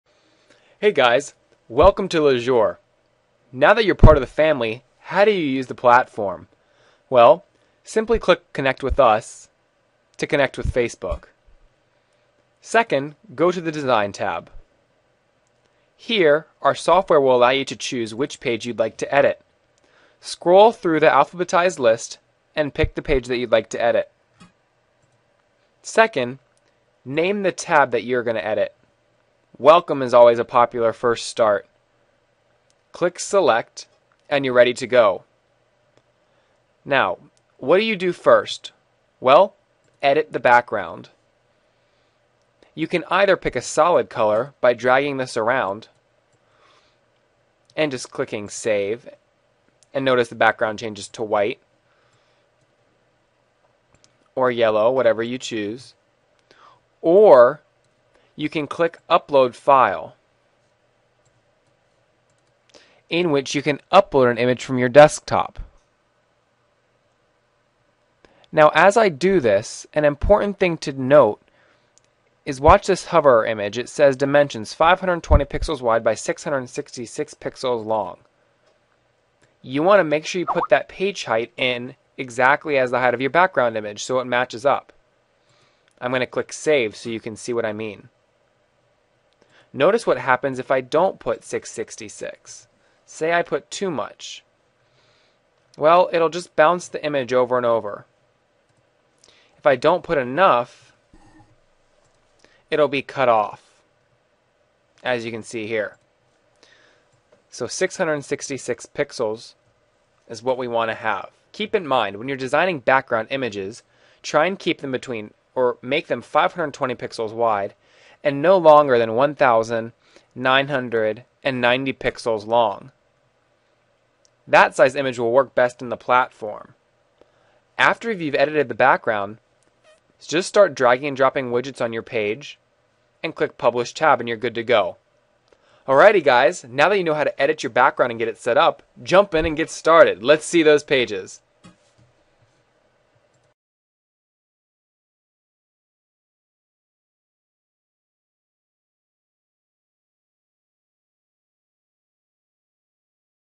How to add a background image to a Facebook Fan Page
This video will walk you through adding a custom background to your facebook fan page in seconds.